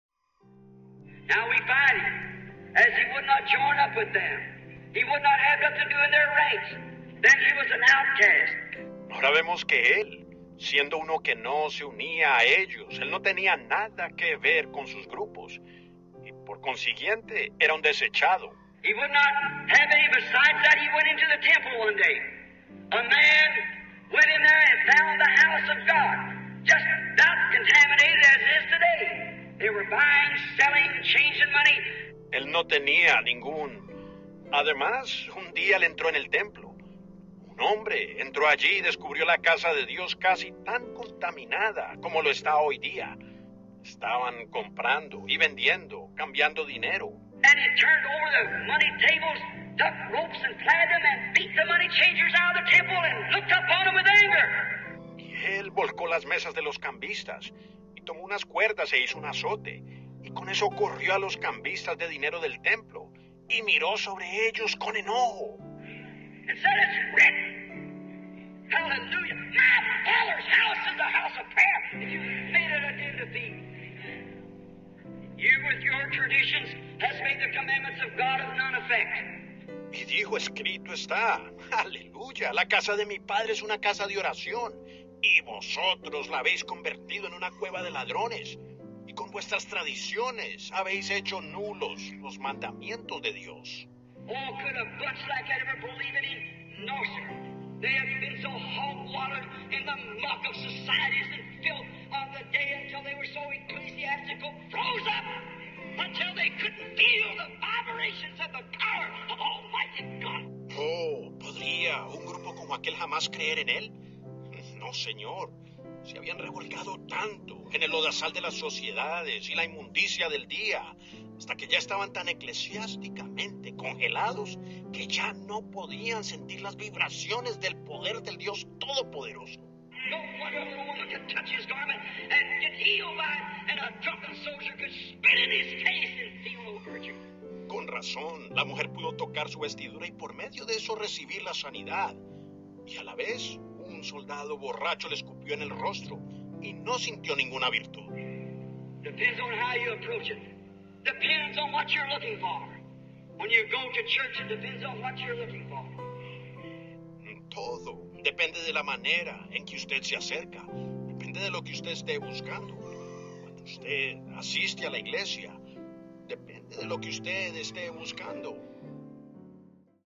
"Hoy Se Ha Cumplido Esta Escritura" Rv. William Branham
65-0219

Hoy Se Ha Cumplido Esta Escritura

Ahora vemos que Él, siendo uno que no se unía a ellos, Él no tenía nada que ver con sus grupos, y por consiguiente era un desechado. Él no tenía ningún...
Además, un día Él entró en el templo. Un Hombre entró allí y descubrió la casa de Dios casi tan contaminada como lo está hoy día. Estaban comprando y vendiendo, cambiando dinero. Y Él volcó las mesas de los cambistas; y tomó unas cuerdas e hizo un azote, y con eso corrió a los cambistas de dinero del templo. Y miró sobre ellos con enojo, y dijo: “Escrito está” aleluya, “la casa de Mi Padre es una casa de oración y vosotros la habéis convertido en una cueva de ladrones. Y con vuestras tradiciones habéis hecho nulos los mandamientos de Dios”.
Oh, ¿podría un grupo como aquel jamás creer en Él? No señor. Se habían revolcado tanto en el lodazal de las sociedades y la inmundicia del día, hasta que ya estaban tan eclesiásticamente congelados que ya no podían sentir las vibraciones del Poder del Dios Todopoderoso. Con razón la mujer pudo tocar Su vestidura y por medio de eso recibir la sanidad; ¡y a la vez un soldado borracho le escupió en el rostro y no sintió ninguna virtud! Todo depende de la manera en que Ud. se acerca. Depende de lo que Ud. esté buscando. Cuando Ud. asiste a la iglesia, depende de lo que Ud. esté buscando.

📖 Pan diario

Isaías 53:7

Angustiado él, y afligido, no abrió su boca; como cordero fue llevado al matadero; y como oveja delante de sus trasquiladores, enmudeció, y no abrió su boca.